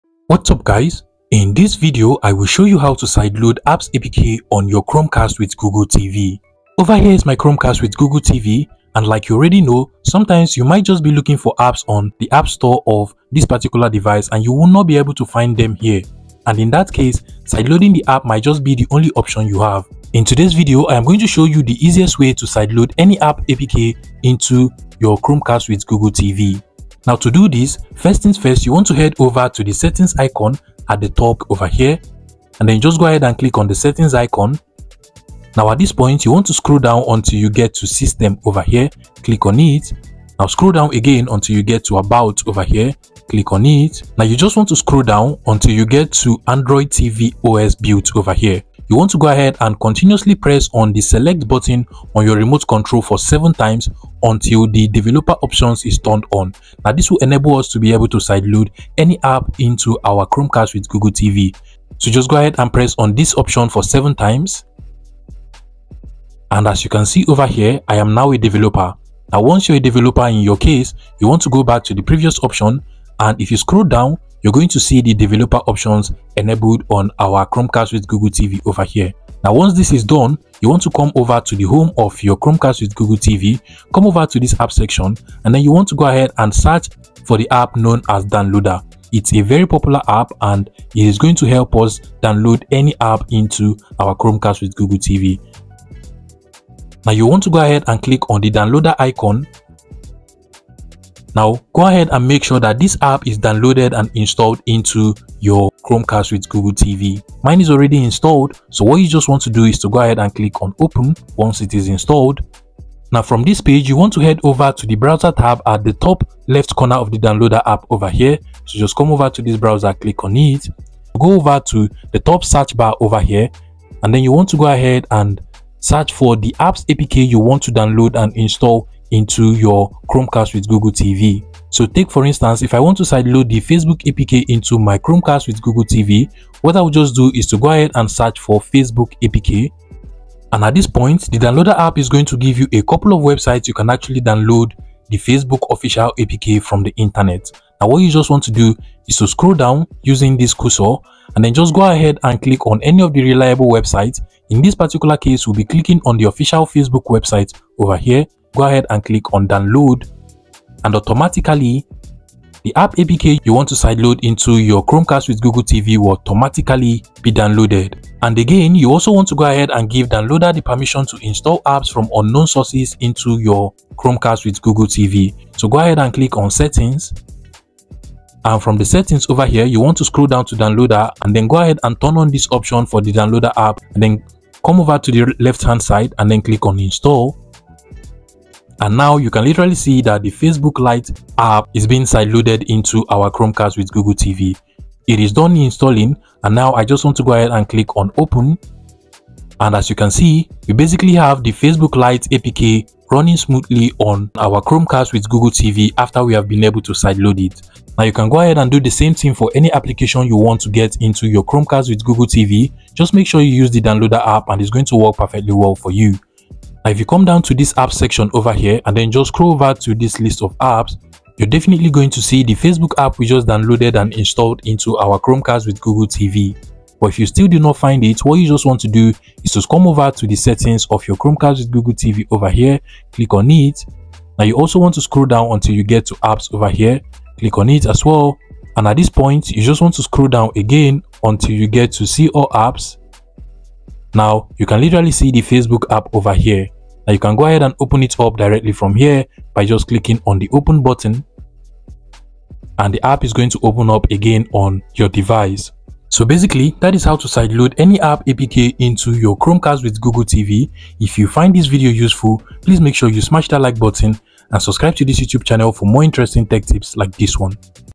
How to Sideload Apps APK on Chromecast with Google TV
In this video, I demonstrated how to sideload apps APK on Chromecast with Google TV. I walked you through the entire process of installing apps that aren’t available on the official app store, so you can get any app you want on your Chromecast. First, I showed you how to enable developer mode and allow the installation of apps from unknown sources. Then, I explained how to download APK files from the internet directly to your Chromecast with Google TV and install them onto the device. Whether you’re looking to install third-party apps or access content not offered on the Google TV store, this guide made the process simple and straightforward.